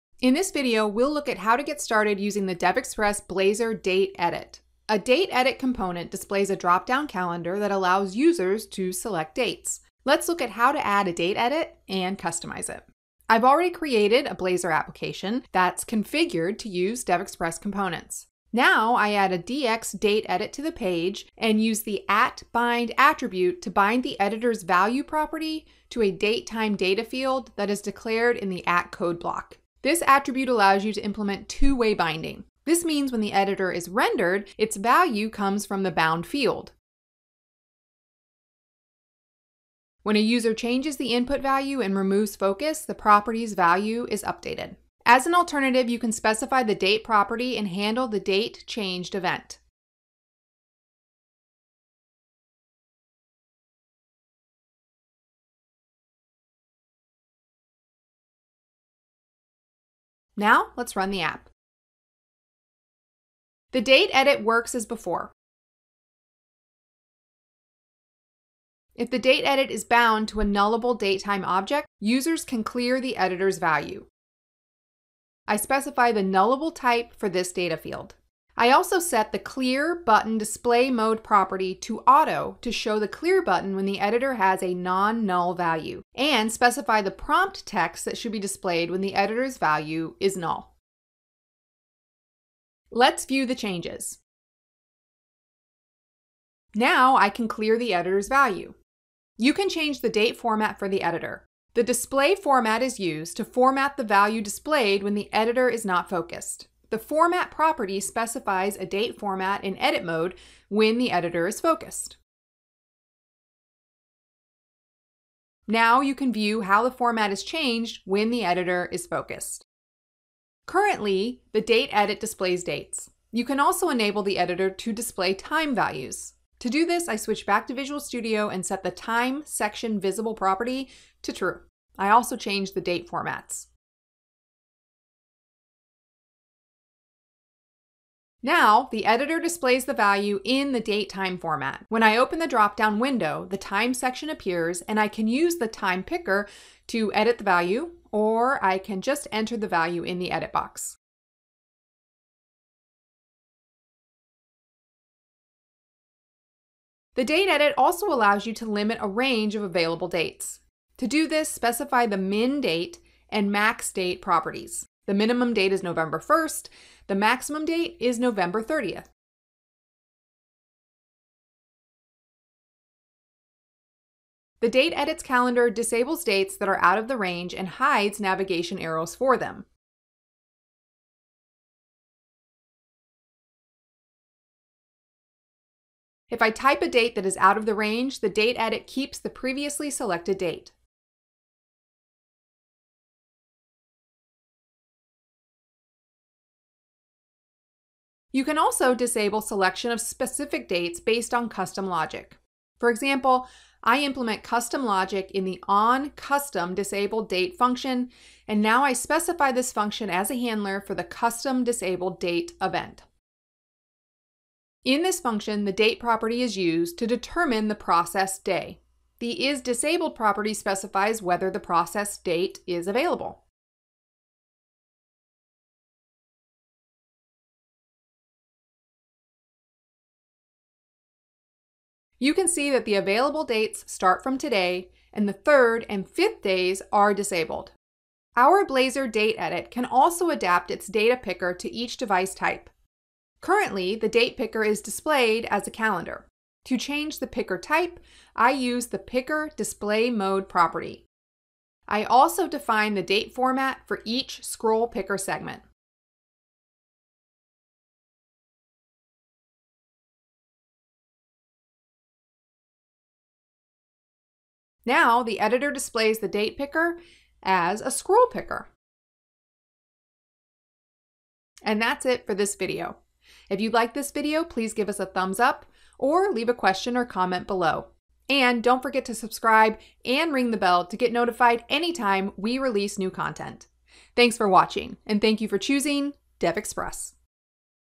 Blazor Date Edit: Get Started
This tutorial demonstrates how to add the DevExpress Blazor DateEdit to an application. The DateEdit component is a date editor with a drop-down calendar or adaptive scroll picker. The component allows you to enter a date into the text box or pick one from the drop-down window.

⏱ Timeline ⏱
0:00 Add a Date Edit to a project and bind it to a DateTime data field
1:18 Show the Clear button and specify a placeholder
1:39 Change the date format for the editor
2:00 Allow the editor to display time values
2:37 Limit the range of available dates
3:18 Disable selection of specific dates based on custom logic
2:58 Change the data picker type